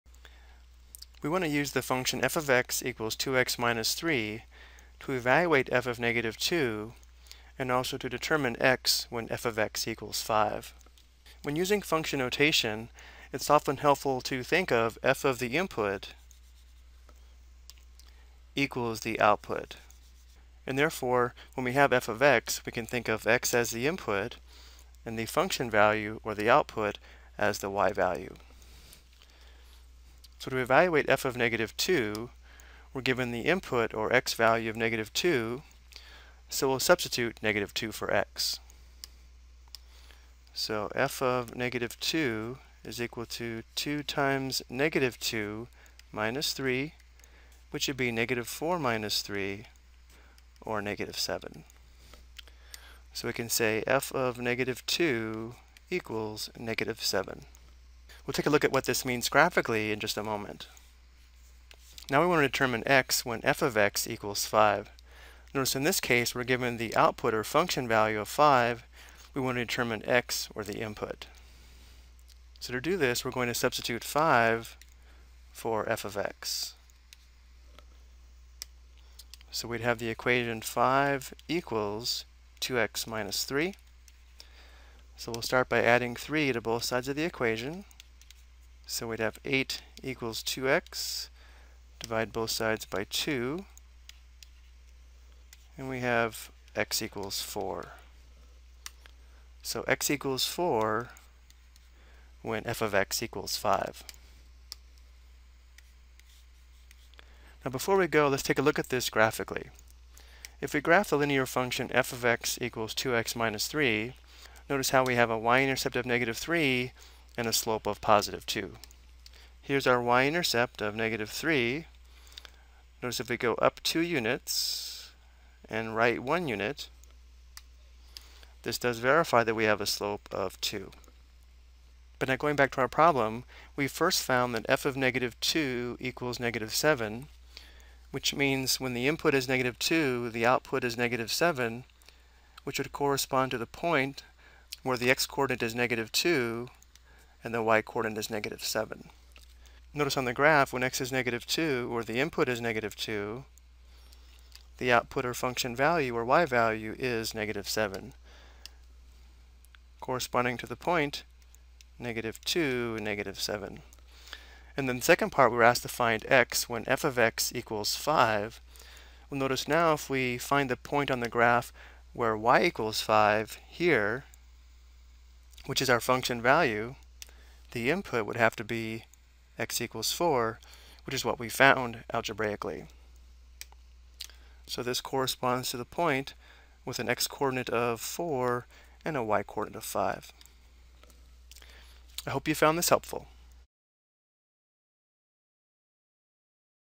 Ex: Determine Function Inputs and Outputs Given a Function Rule
This video provides examples  of how to find function inputs and outputs when given a function a s set or points or ordered pairs.